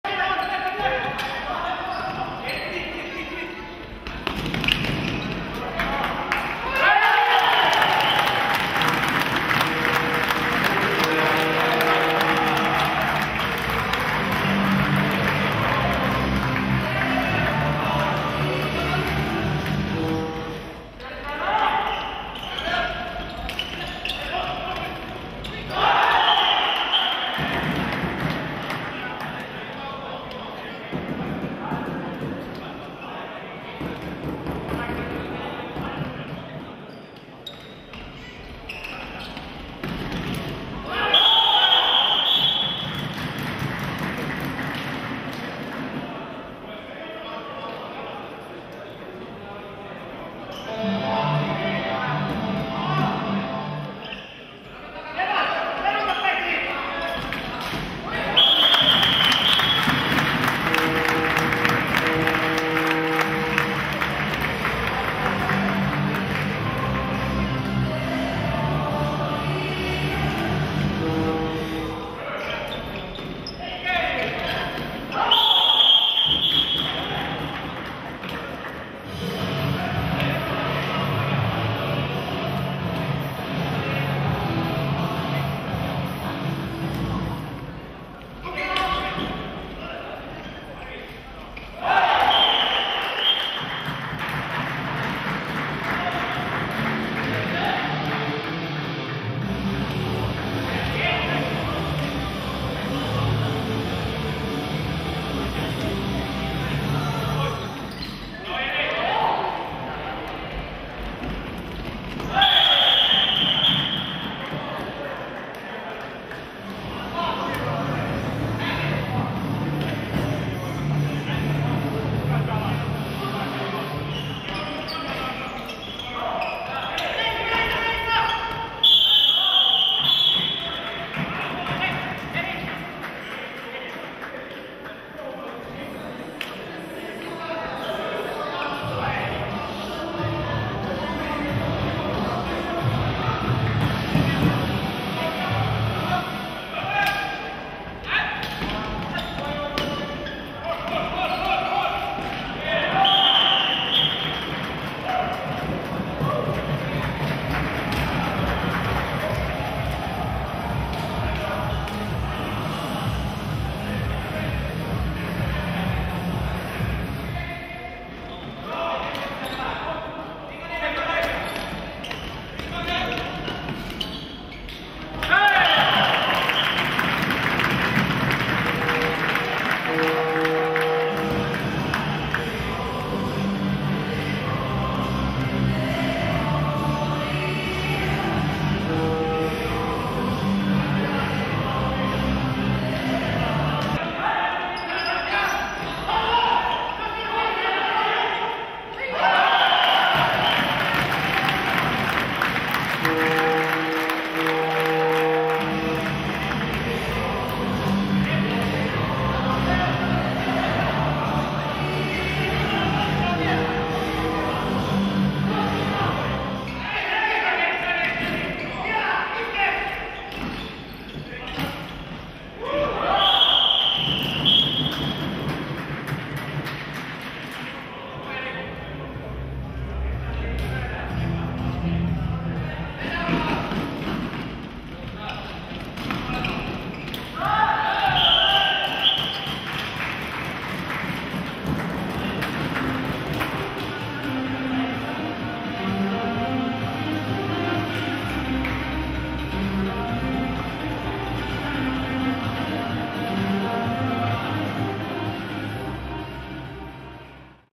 Blue Fox - Umeå City la 18.8.2012
Blue Fox -Umeå City la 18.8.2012. Harjoitusottelu Uudenkaarlepyyn Tähtihallista.